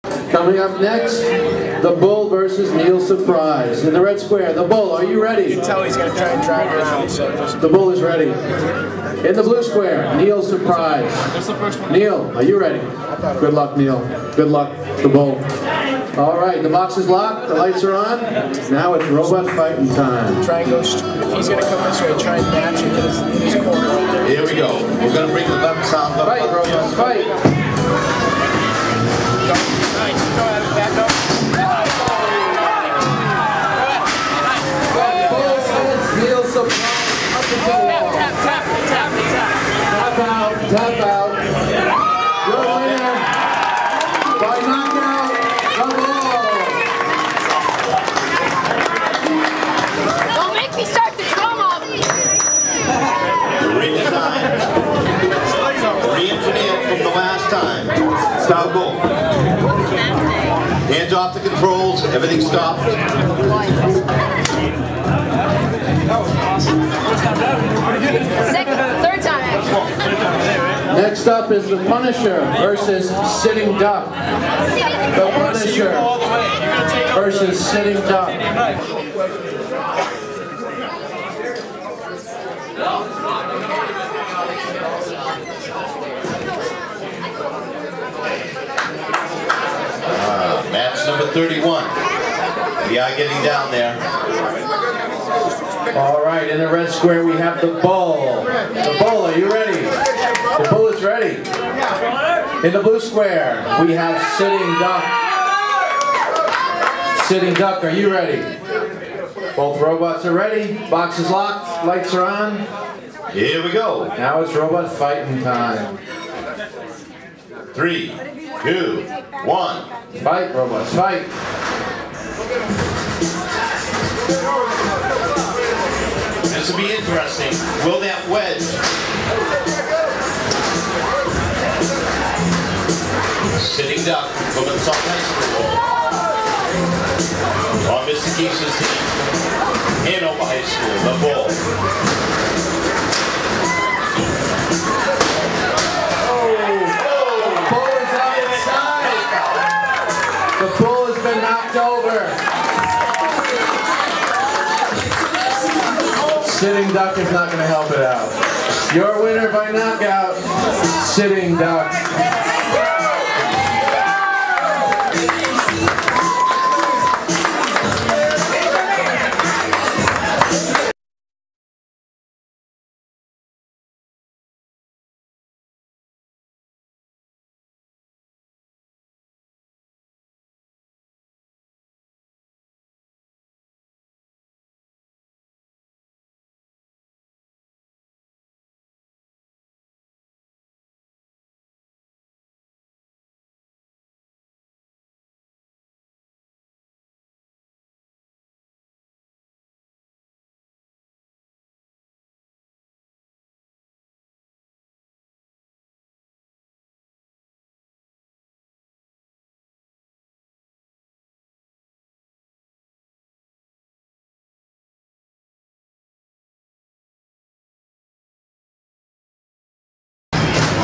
Battlebots IQ Rumble At The Rock March 2009 Hanover High
Watch the Hanover High robitics team "Rumble at the Rock" at Plymouth North High School Saturday, March 21.